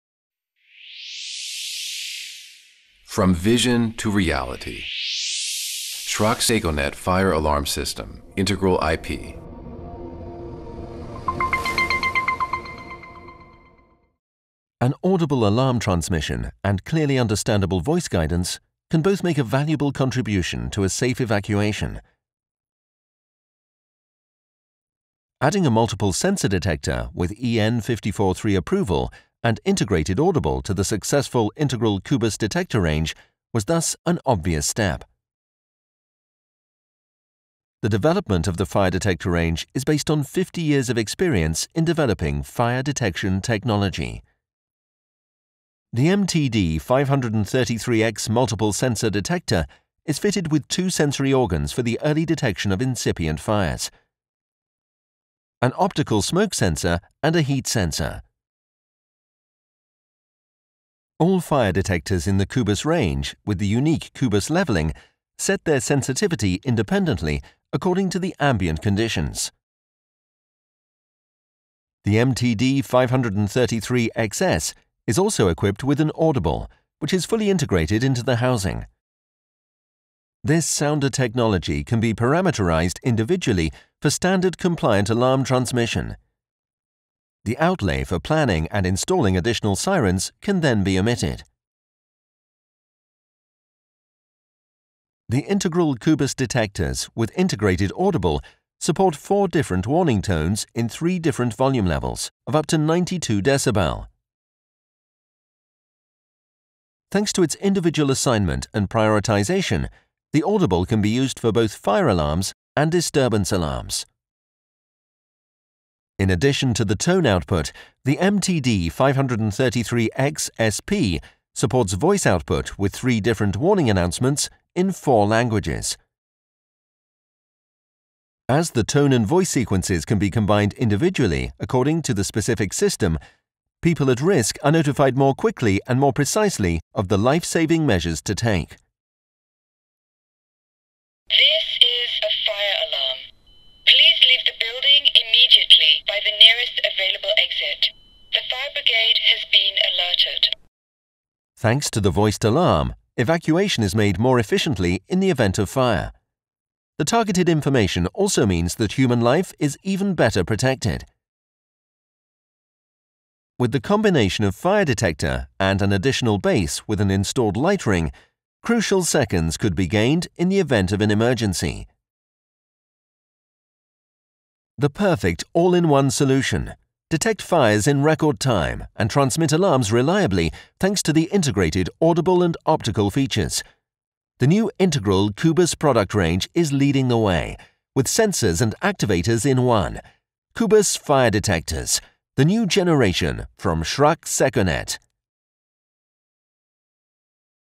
Schrack Seconet - Multiple Sensor Detector MTD 533X-S-SP - english
The perfect all in one solution – detect fires in record time and transmit alarms reliably thanks to the integrated audible and optical features. The new Integral CUBUS product range is leading the way. With sensors and activators in one.